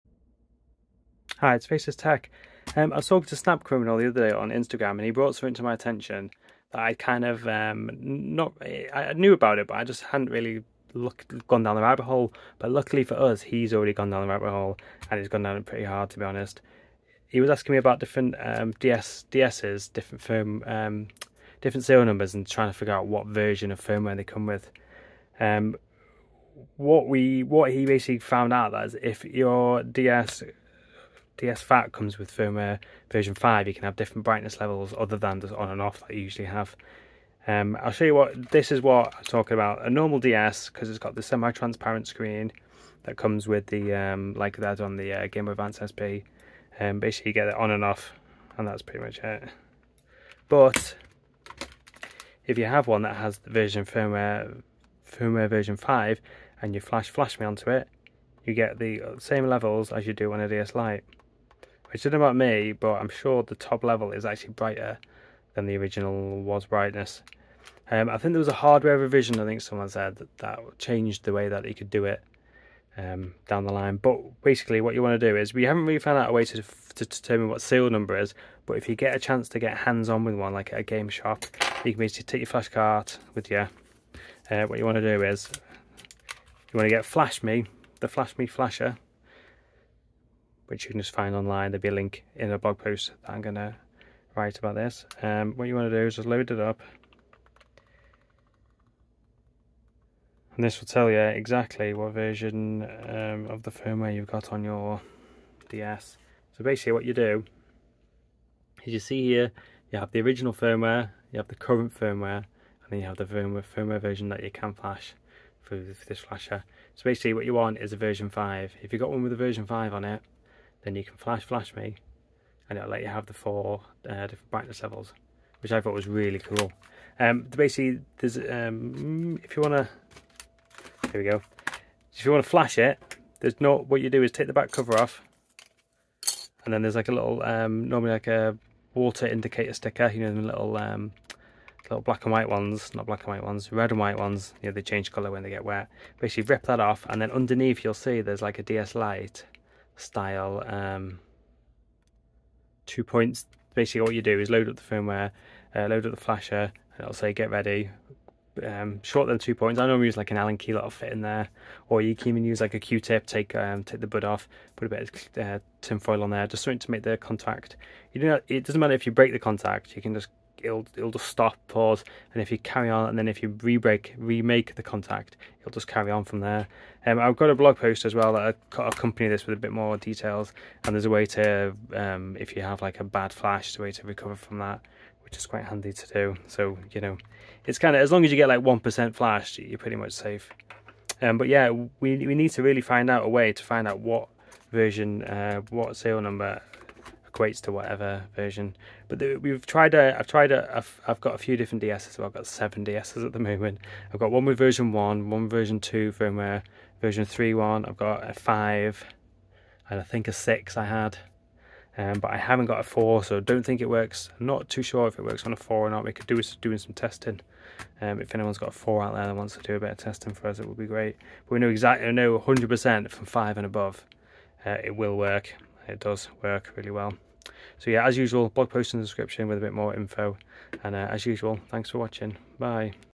DS Phat multi brightness levels
If your ds phat came with version 5 firmware or above it can be flashed with flashme v8 to unlock the ability to for multi level brightness like the ds lite. You can check the firmware versions vi the flashme app. More info about flashing can be found in the blog post below.